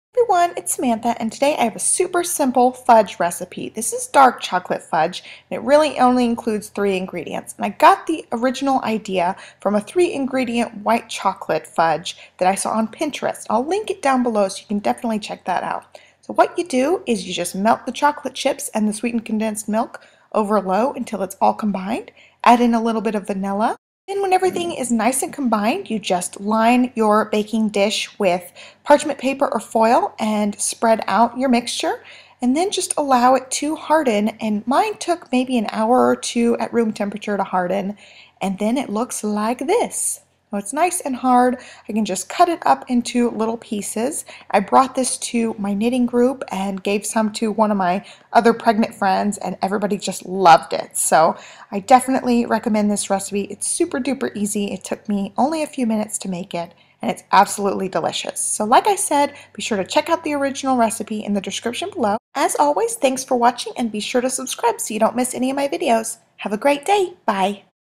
Easy Cheater Fudge Recipe | Just 3 Ingredients!
Ingredients:
1 & 1/2 BAGS of dark chocolate chips (18 oz total)
8 oz can of Sweetened Condensed Milk
1 tsp vanilla

Melt chocolate chips and sweetened condensed milk together over low heat until completely melted and smooth. Stir in vanilla. Pour mixture into a baking dish lined with parchment paper or foil. Allow to cool completely & harden before cutting & serving. Can be stored in an airtight container for up to 10 days or in the fridge/freezer much longer.

~*~*~*~*~*~*LINKS*~*~*~*~*~*~

Try out my favorite cleaning products from The Honest Company with a $10 off coupon!